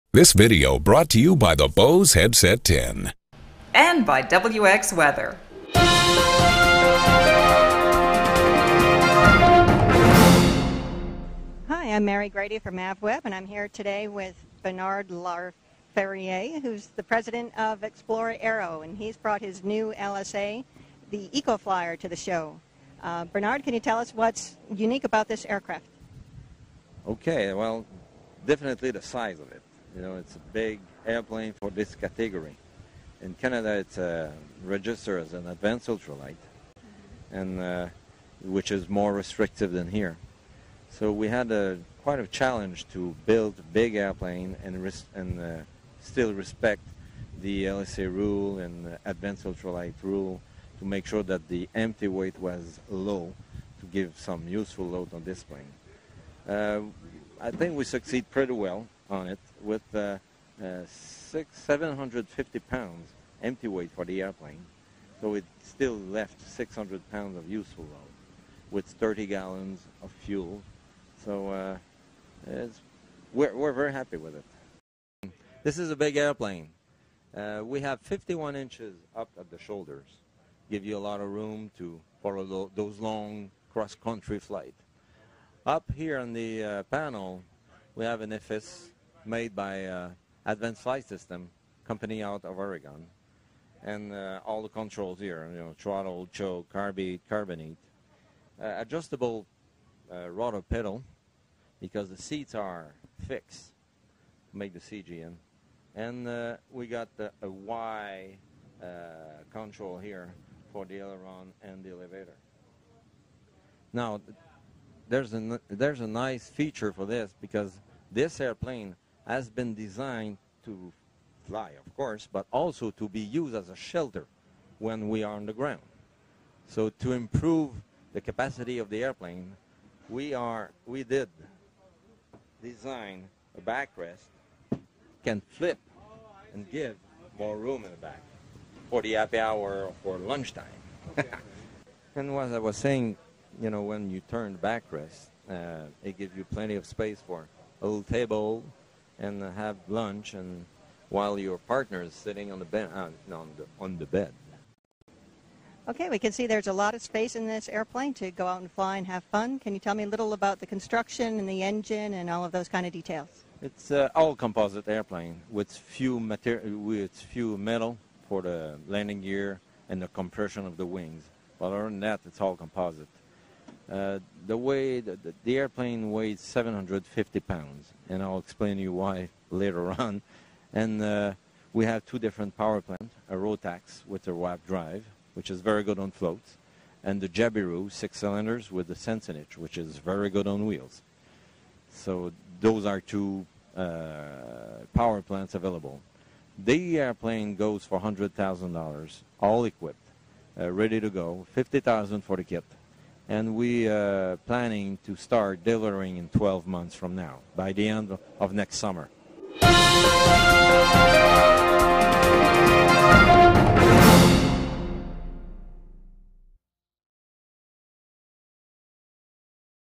EcoFlyer, The Live-In Light Sport Aircraft -- Oshkosh '08
ExplorerAero's Ecoflyer LSA: 130 mph, Flying camper with 1100 ft/min climb and 6 hour endurance for $100,000 ready to fly, according to the company.